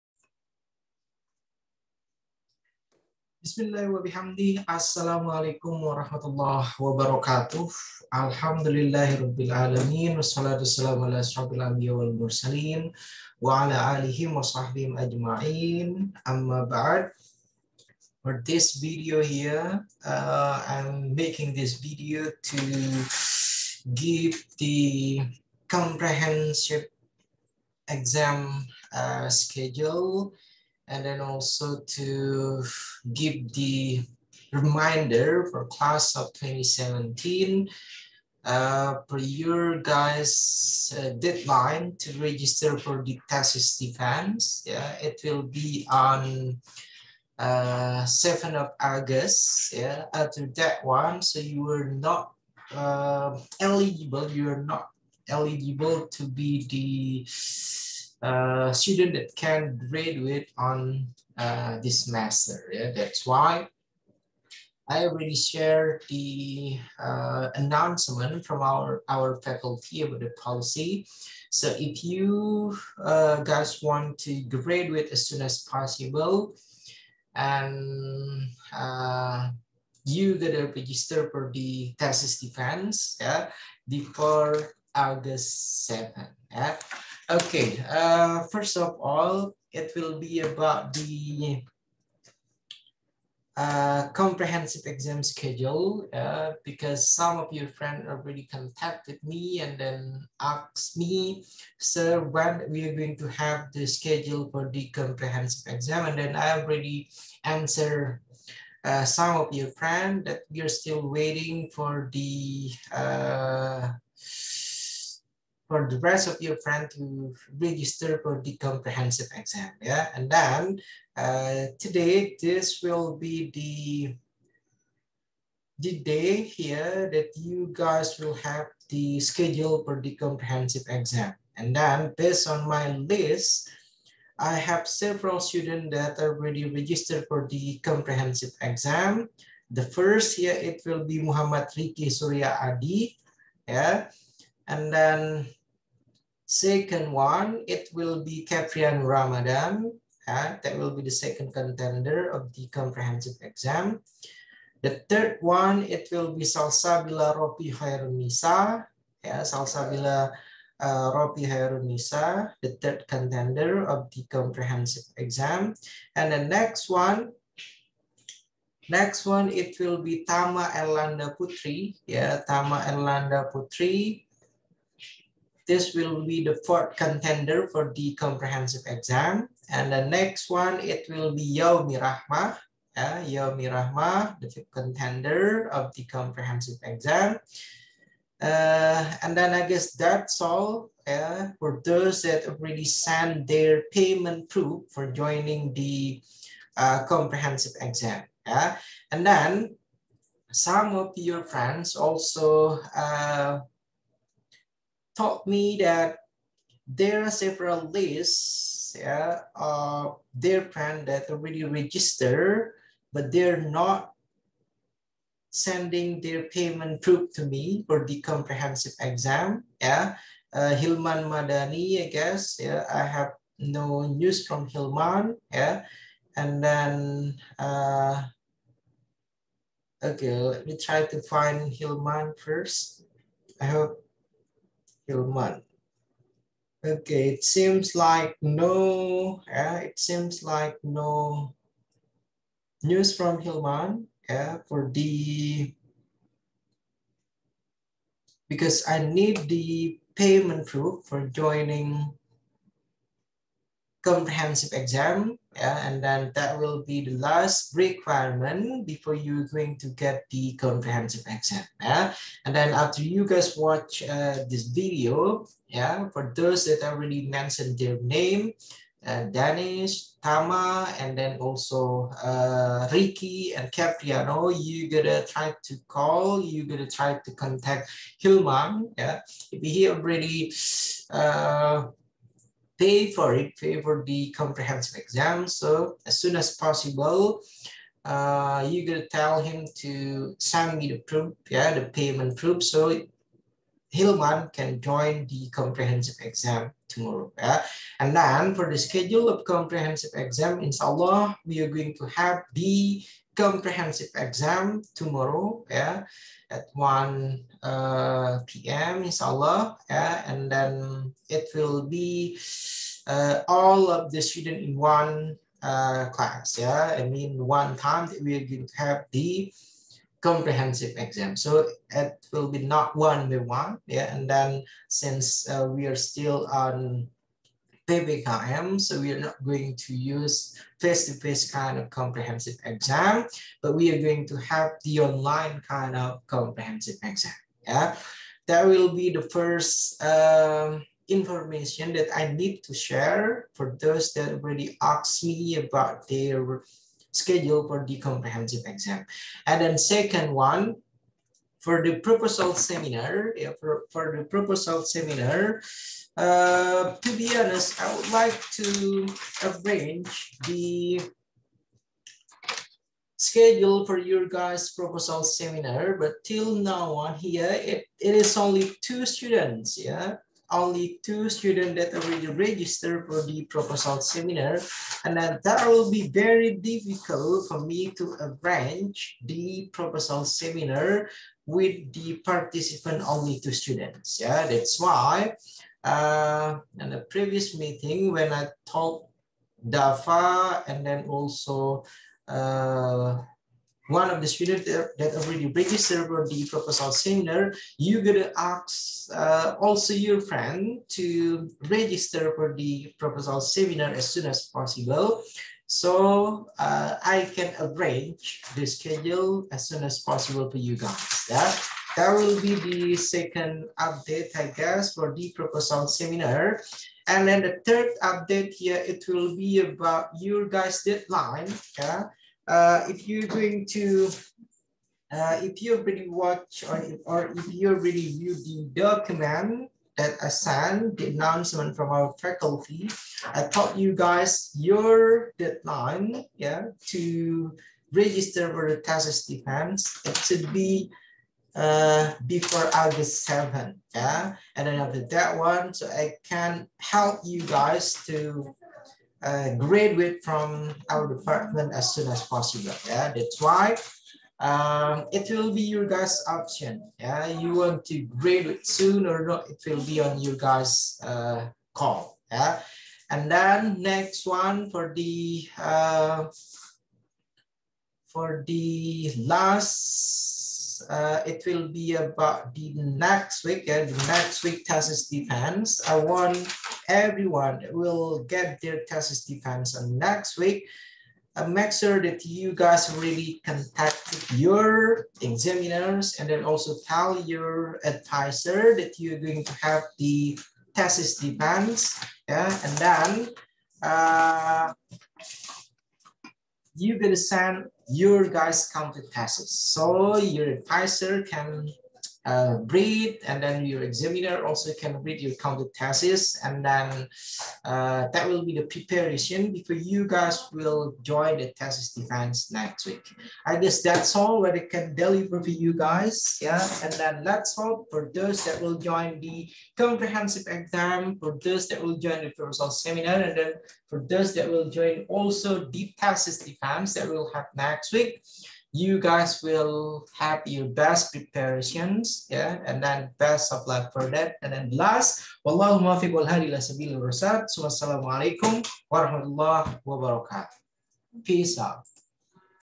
COMPREHENSIVE EXAM SCHEDULE, PROPOSAL SEMINAR UPDATE, THESIS DEFENSE PREPARATION.
Schedule of Comprehensive Exam
Proposal Seminar Update
Thesis Defense Update

English Department
Faculty of Education
Muhammadiyah University of Jakarta

LECTURER : Dr. Muhamad Sofian Hadi, S.S, M.Pd